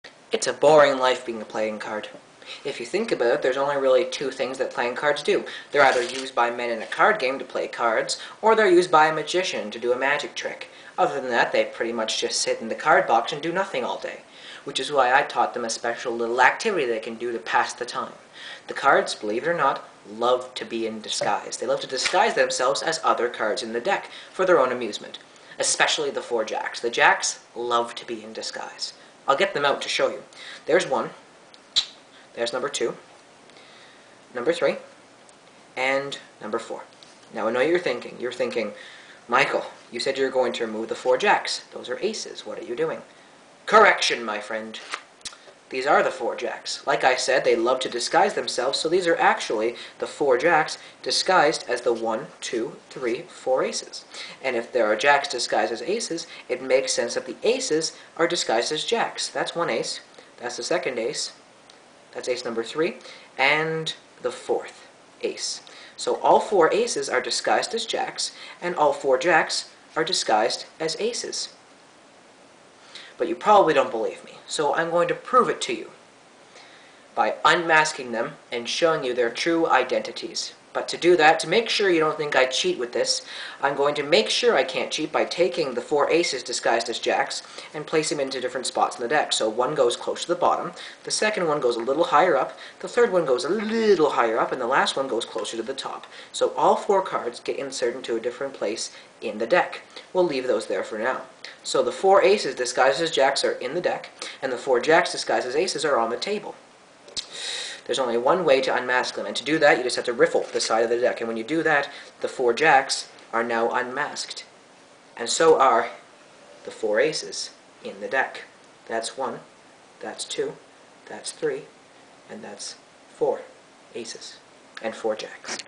JackSyna(ps)ces by Ernest Earick
I perform a brilliant effect by Ernest Earick from his book "By Forces Unseen: The Innovative Card Magic of Ernest Earick" by Stephen Minch. Comments are greatly appreciated. Enjoy!